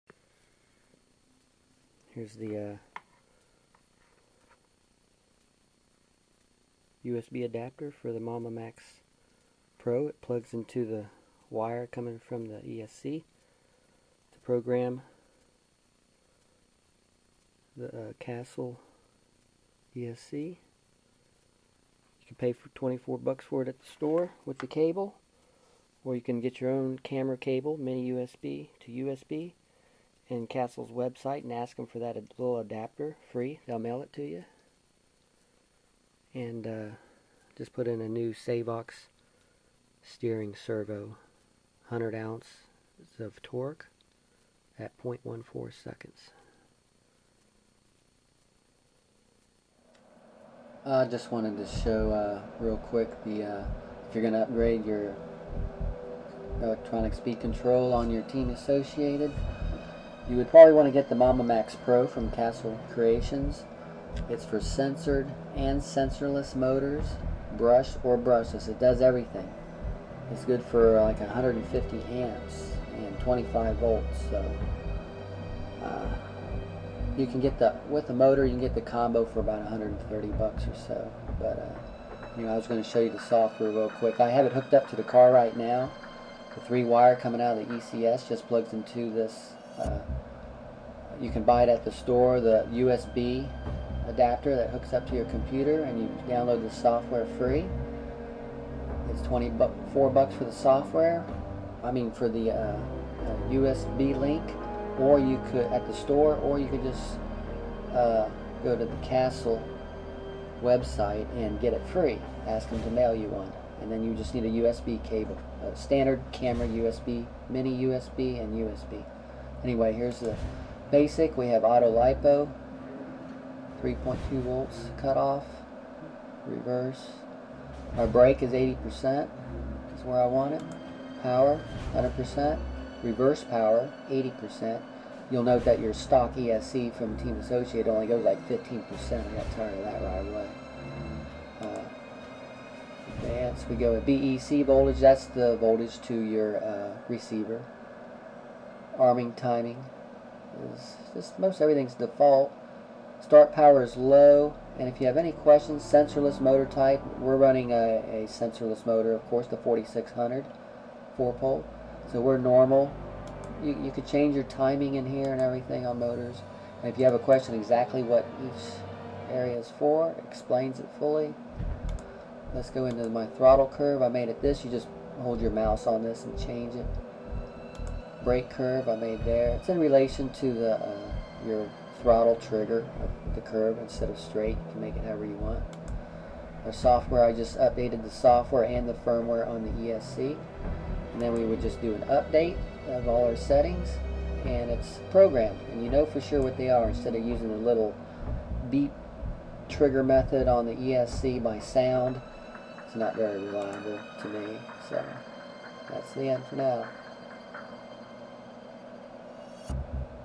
SC10rs with Mamba Max Pro ESC and Castle Link.avi
Mamba Max Pro Castle ESC with smart sense. Visual of software on computer to program ESC. Ohh wile we were under the hood added a Savox Steering servo SC-0254MG 100 oz torque at 0.14 sec. 6v.